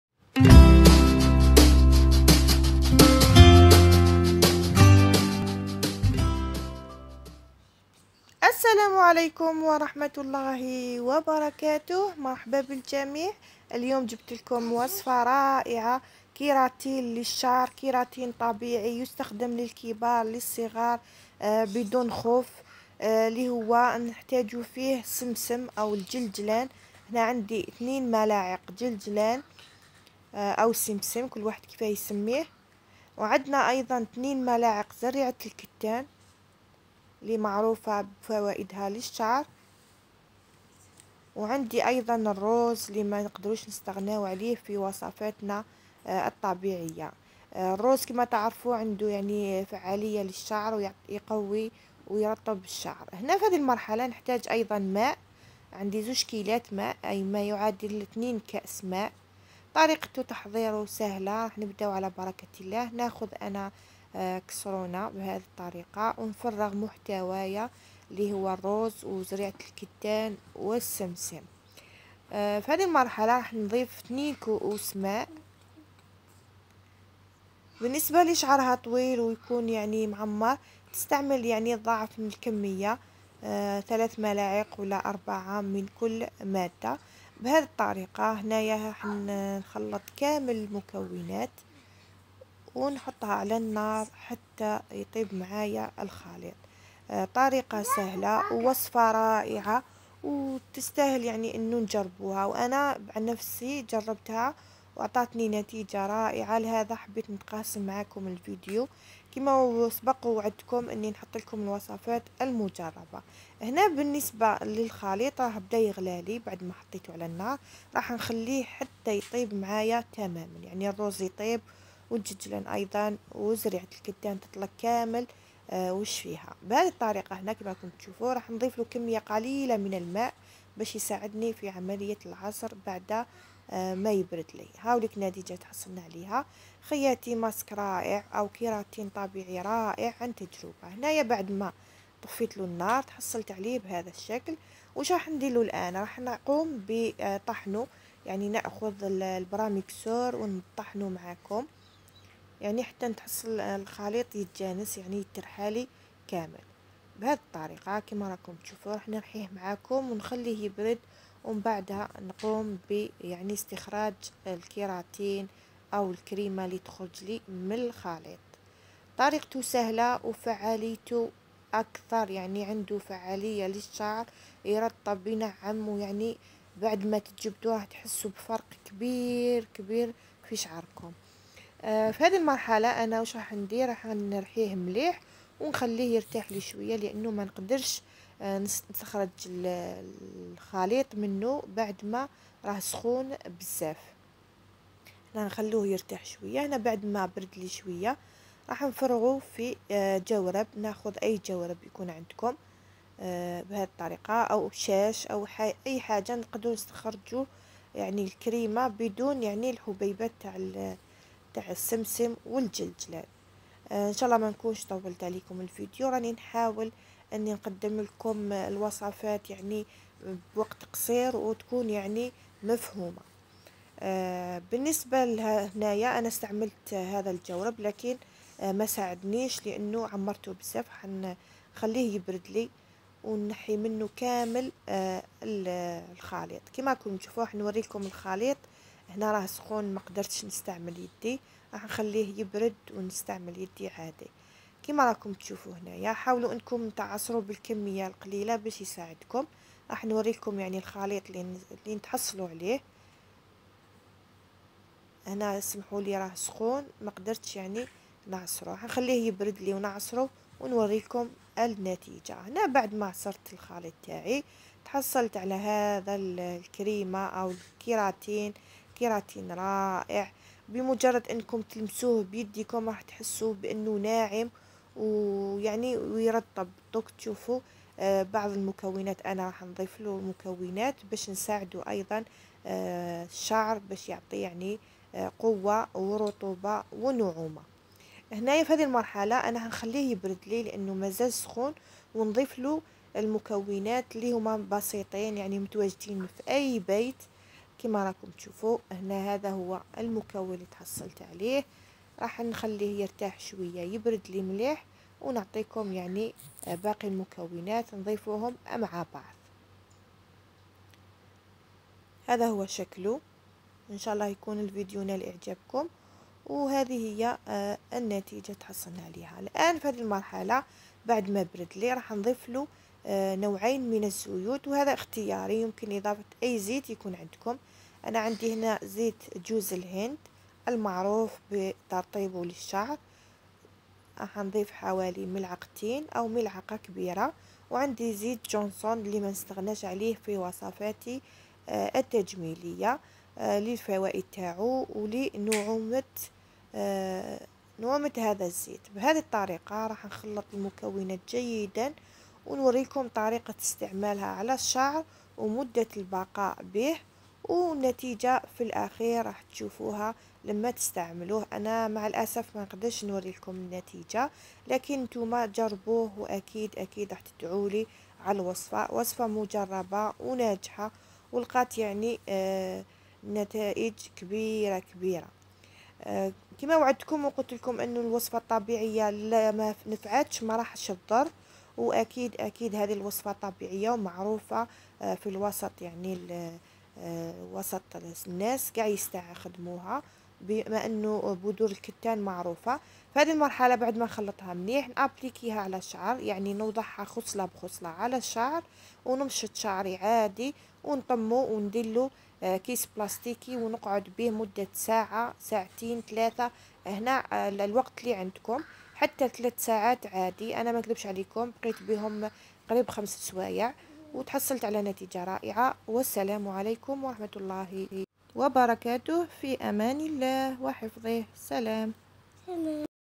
كراتين طبيعي للشعر بمكونات طبيعية مئة بالمية ناجح عن تجربة يرطب الشعر ويرجع يهبل
وصفة الشعر طبيعية تخلي شعرك كالحرير
من انجح الوصفات الطبيعية
خلطة طبيعية رهيبة للشعر ناجحة وتخلي شعرك كالحرير بالسمسم والروز وزريعة الكتان
لقات نجاحا كبير عندي و عند كل نستعملها

زريعة الكتان
روز
سمسم
زيت جوز الهند
زيت جونسون

لقات نتائج رائعة عند مستخدميها من اول مرة
وعن تجربتي الخاصة

المقادير في الفيديووووو مع طريقة عملتها وطريقة استخدمو على الشعر مع مدة البقاء بيه على الشعر
وصفة تهبل راح تجربوها وتدعولي عليها

لاتنسووووو البصمات 👍👍👍 والاشتراك بالقناة وتفعيل الجرس 🔔🔔🔔🔔🔔🔔🔔🔔🔔🔔🔔🔔 والبرطاج مع من تحبون